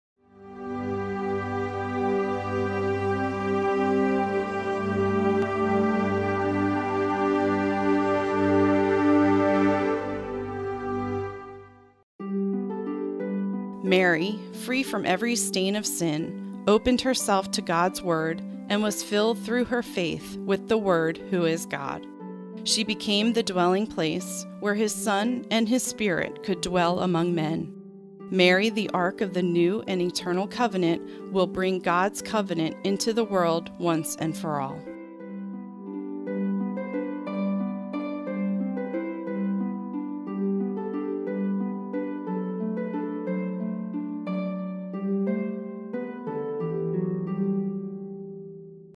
Mary the first tabernacle
Mary, free from every stain of sin, opened herself to God’s word and was filled through her faith with the Word who is God. She became the dwelling place where his Son and his Spirit could dwell among men. Mary the Ark of the New and Eternal Covenant will bring God’s covenant into the world once and for all.
Recite the Angelus punctually at noon and with great affection.

Read more about the Year of Mary on The Catholic Miscellany’s